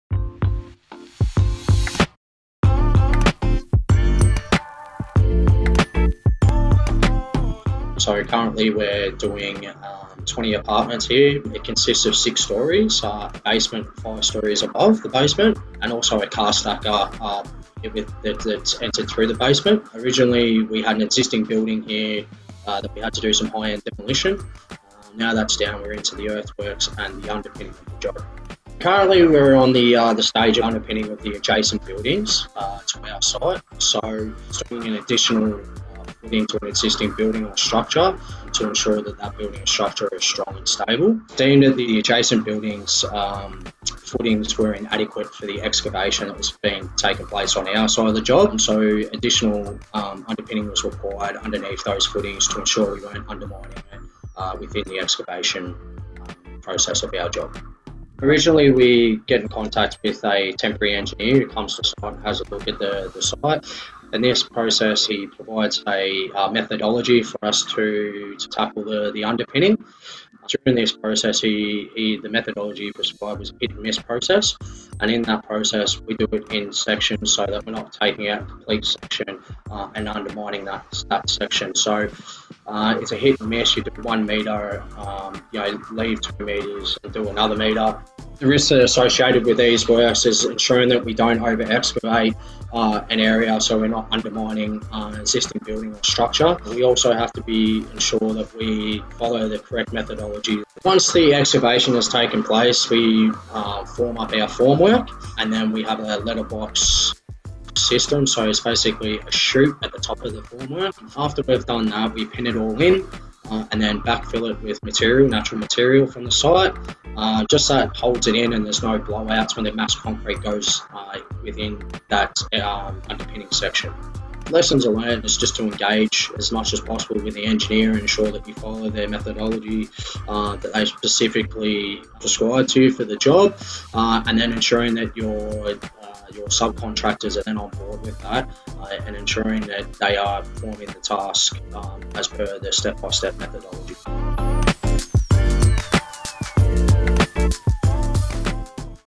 Construction Insight Series #2 - Underpinning
Whilst the practice may not be widely understood, underpinning is an important requirement for the protection of building, foundations and/or adjoining properties. Watch this episode to find out more.